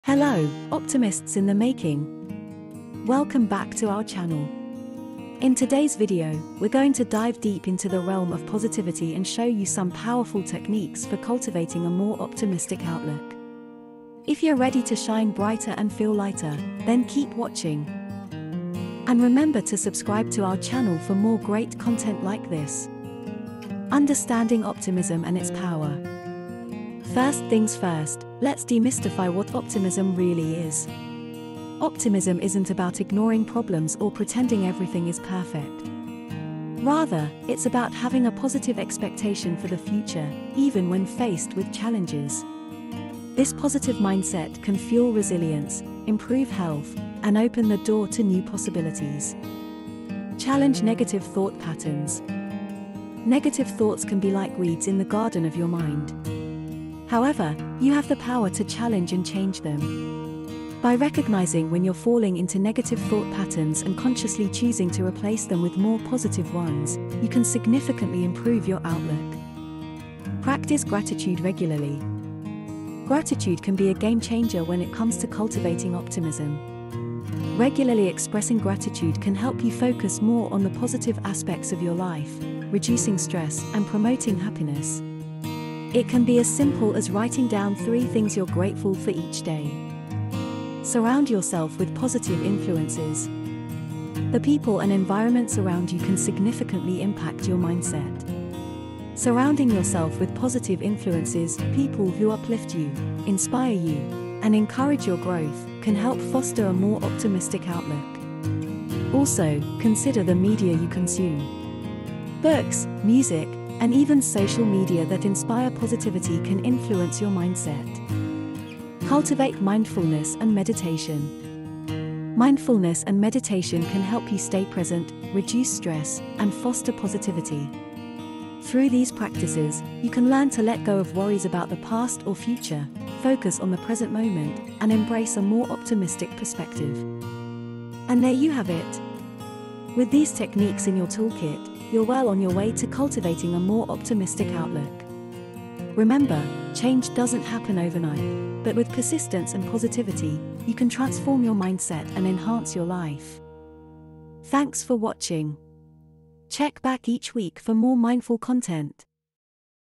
Techniques For Cultivating A More Optimistic Outlook
Welcome to our in-depth guide on Techniques for Cultivating a More Optimistic Outlook😊🌻. In this video, we'll explore powerful ways to increase your positivity and build a stronger, more optimistic mindset. From understanding the power of optimism, challenging negative thought patterns, practicing regular gratitude, surrounding yourself with positive influences to cultivating mindfulness and meditation, we've got you covered! Let's embark on a journey towards a brighter, happier life together.

Ways To Cultivate Optimism (Credit: Kripalu)

Tips To Be More Optimistic (Credit: Psychology Today)

🌼 Remember: A positive mindset is the first step towards a brighter future! 🌈